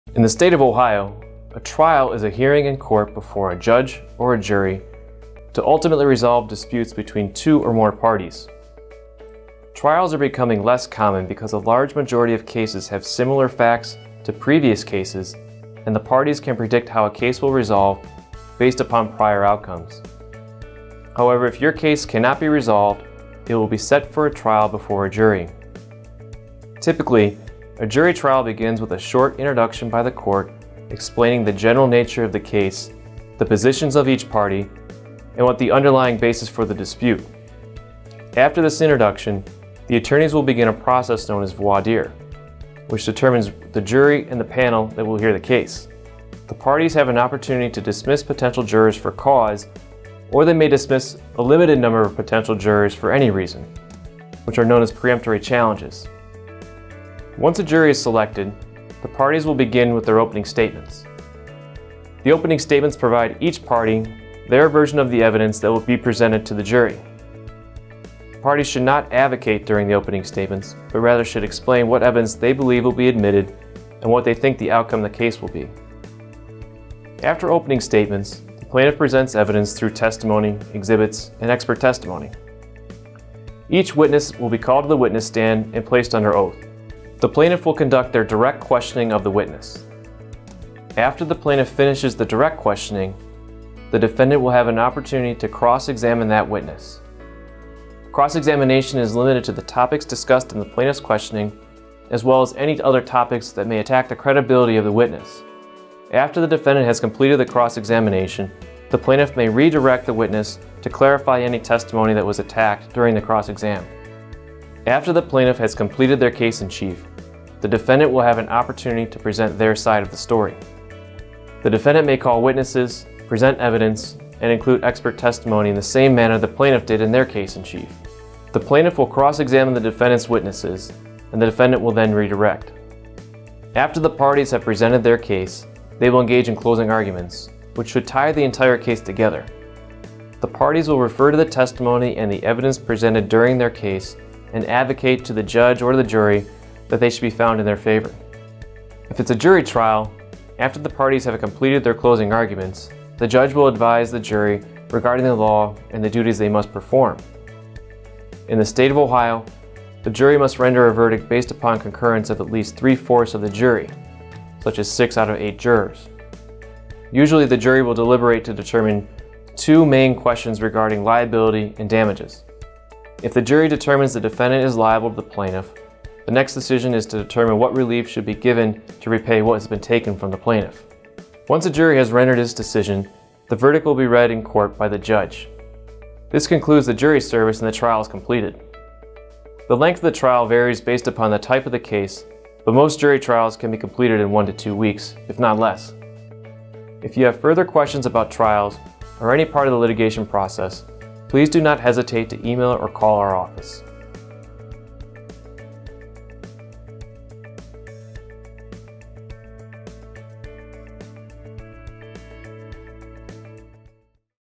What to expect with your court case (Part 6 of 6) – Trials in Ohio
So how dose a trial actually proceed. Thomas Ryan of Ryan LLP gives us the rundown on how a court trial usually goes.